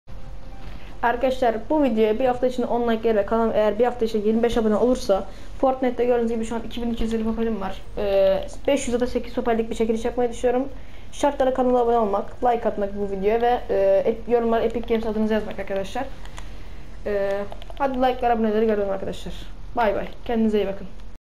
Fortnite Çekiliş Duyuru !!!
1 hafta icinde 25 abone bu videoya 10 like ve yorumlara epic games adı ve katılıyorum yazmak Nick:GorkemGokel